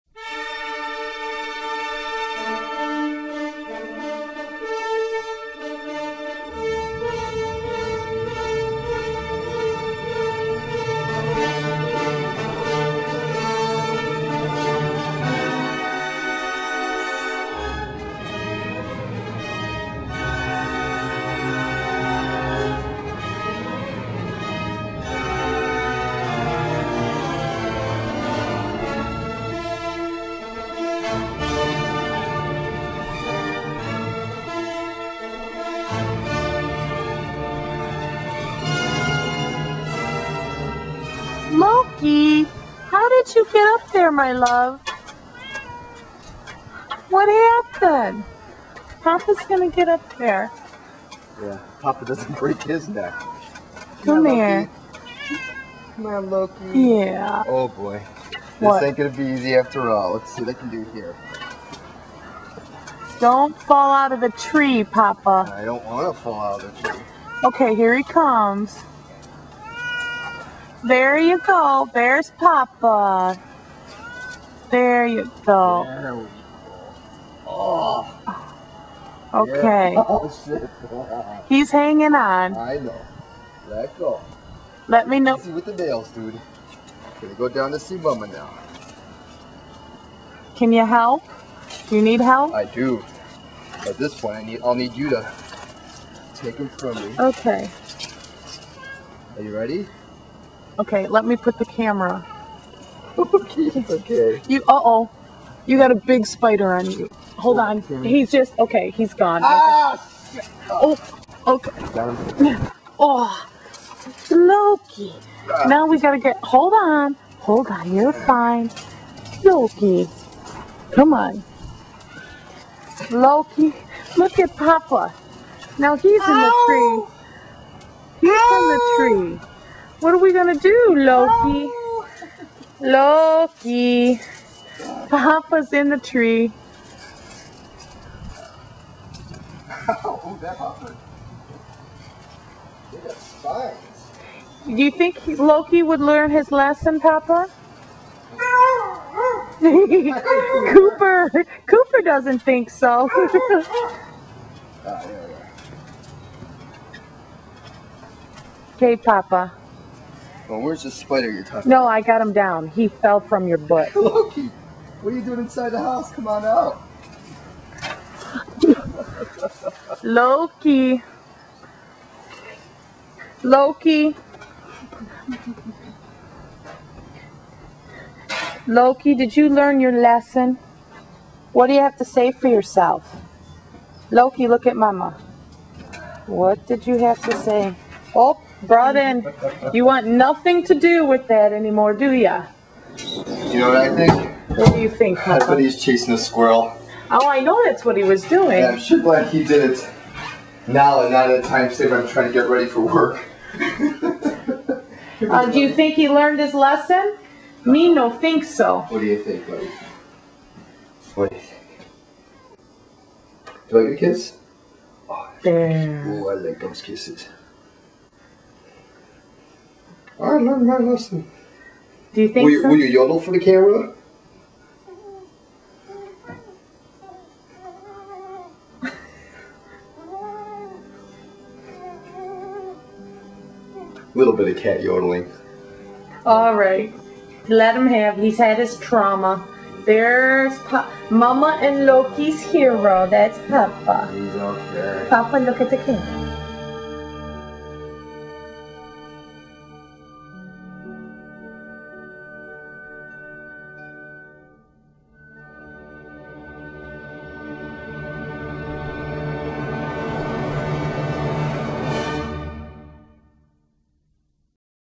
Loki stuck in a tree
Last summer, our cat Loki, got stuck in a tree. Unfortunately for him, but lucky for us, the whole rescue adventure was captured on live camera by my wife. And to top it off, we even got some cat yodeling out of the big guy.